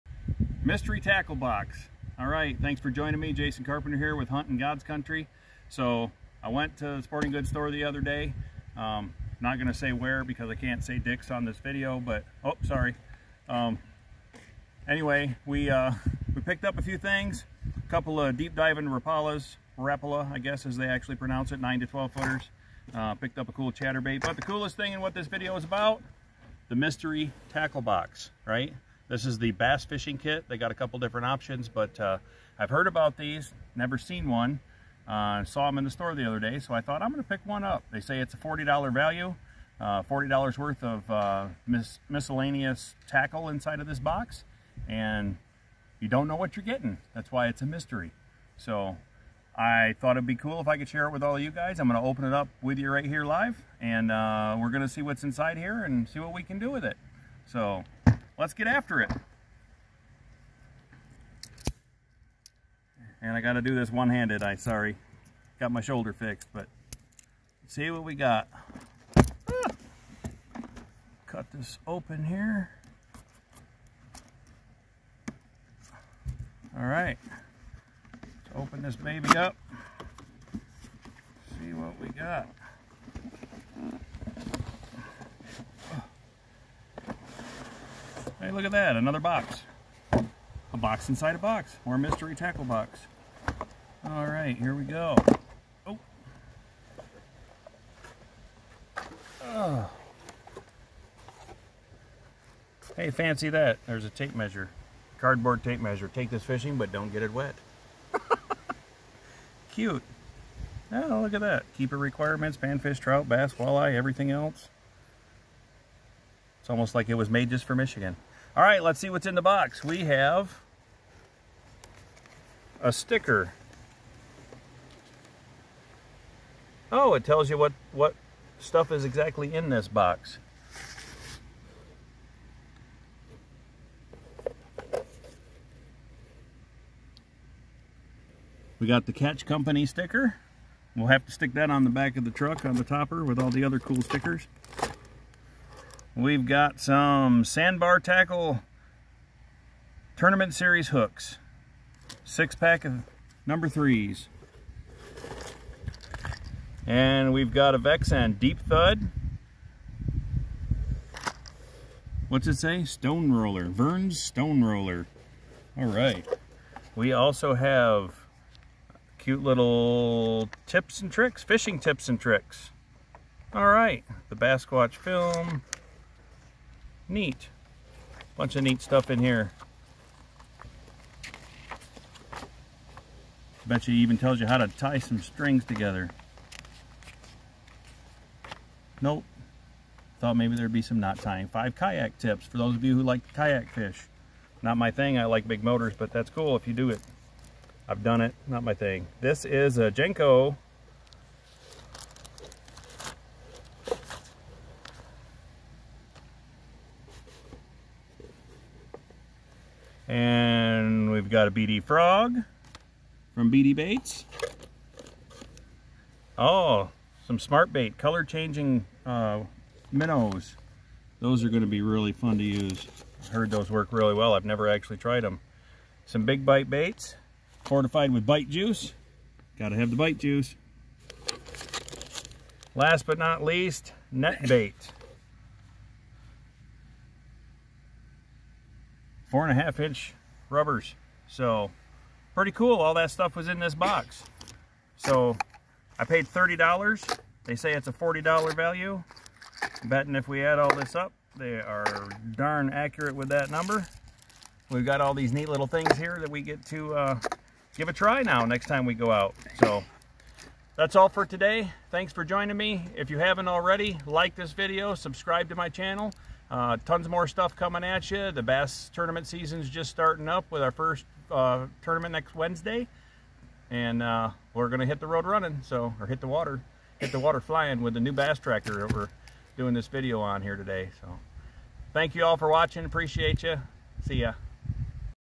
I Bought A Mystery Tackle Box!
I bought a mystery tackle box and opened it up to see what's inside it.